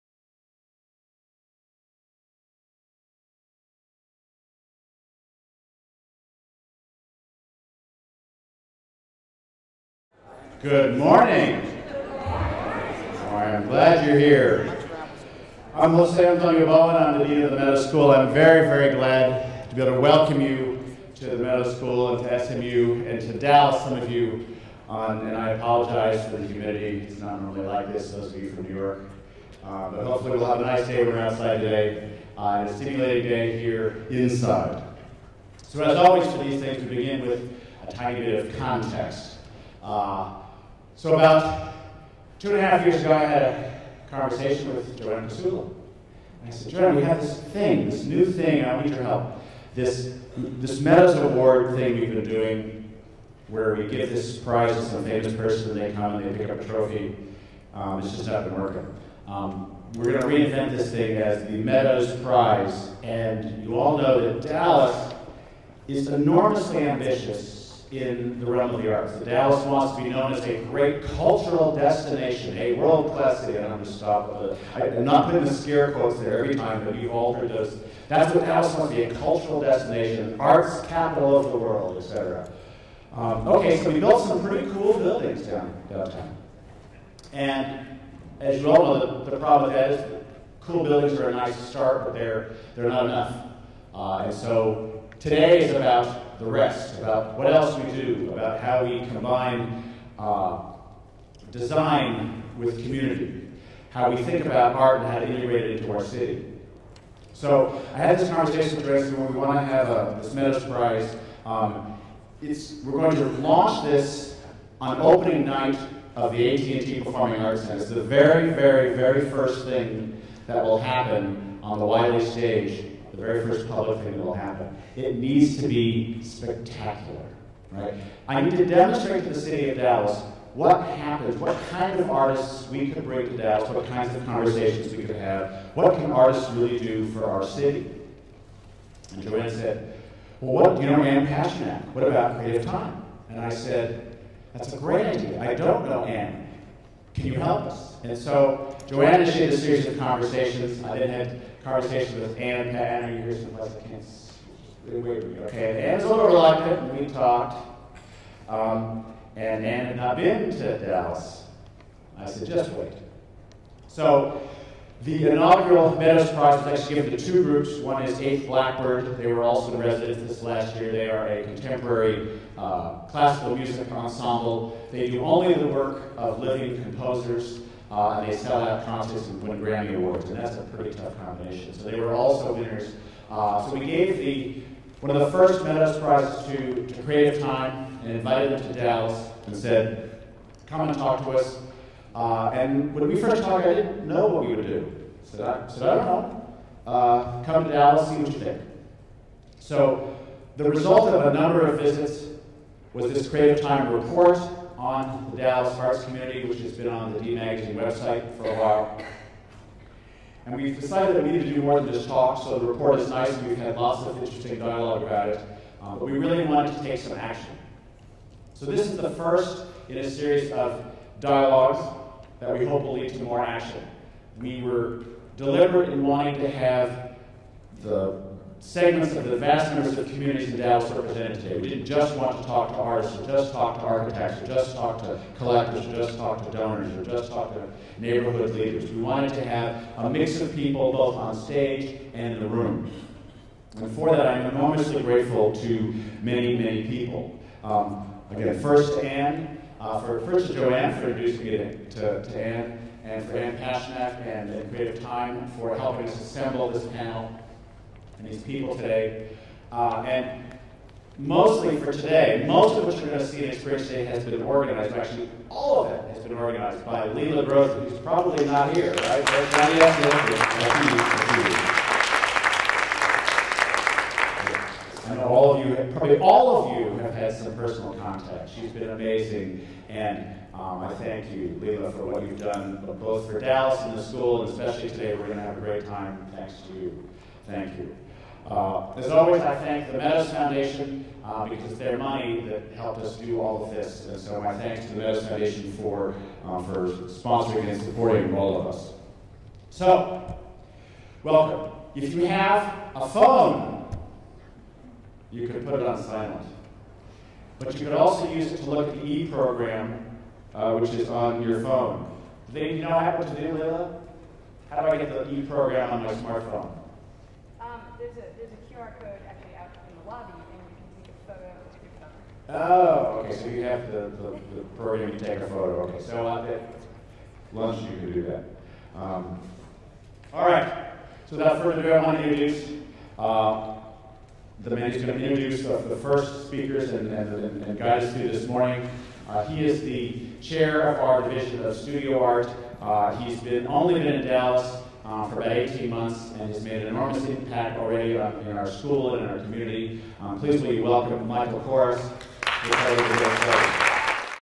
Freedom of the City Symposium: Introduction from SMU Meadows Dean Jose Bowen
The Freedom of the City: Models of Urban Engagement & Creativity in the 21st Century at SMU Meadows School of the Arts in Dallas, TX

Opening remarks by José Antonio Bowen, Dean of Meadows School of the Arts, SMU.

Welcoming the audience to our conference, Dean José Bowen provides a context for the event by describing our Meadows Prize, the Creative Time Residency and Report, and highlighting our interest in understanding and constructing in Dallas new relationships between urban development, architecture, art, and community.